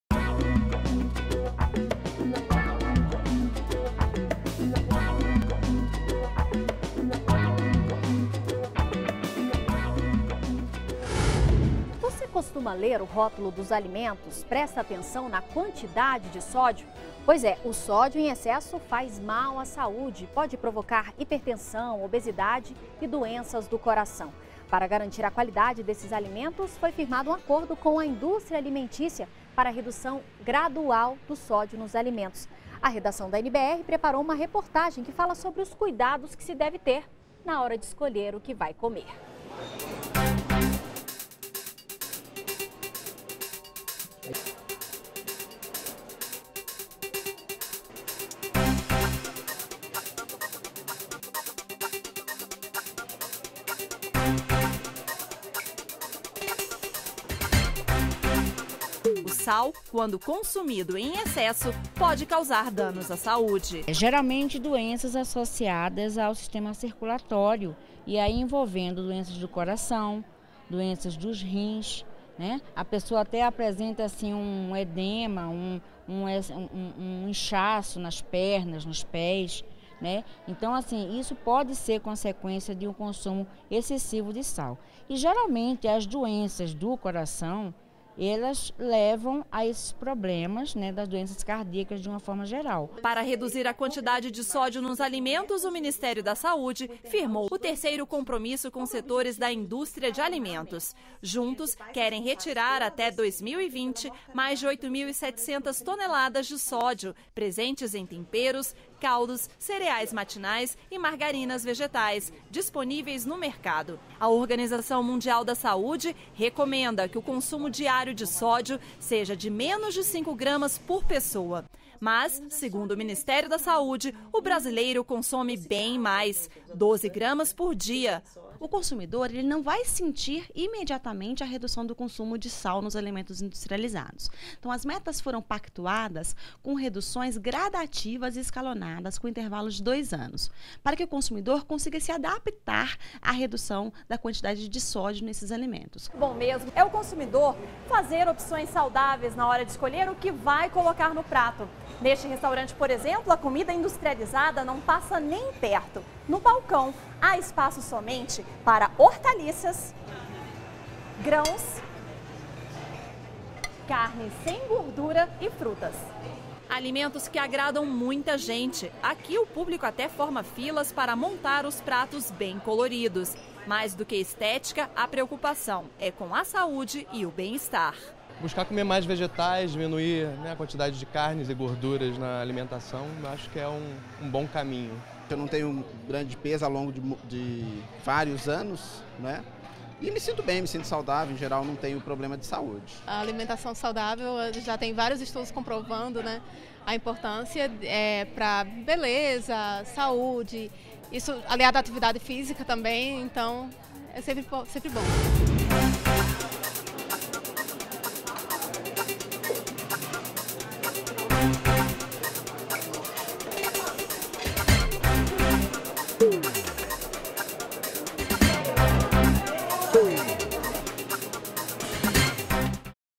Governo firma acordo com indústria alimentícia para redução de sódio nos alimentos
DA REDAÇÃO - 04.09.12: O sal em excesso faz mal à saúde. Pode provocar hipertensão, obesidade, além de doenças do coração. Para garantir mais qualidade nos alimentos, o governo firmou acordo com a indústria alimentícia para redução do sódio nos alimentos. É recomendado pela Organização Mundial da Saúde (OMS) o consumo diário de, no máximo, 5g de sódio por pessoa. No entando, dados do Ministério da Saúde (MS) mostram que brasileiro consome cerca de 12g por dia. O MS pretende retirar do mercado quase 9 mil toneladas de sódio até 2020.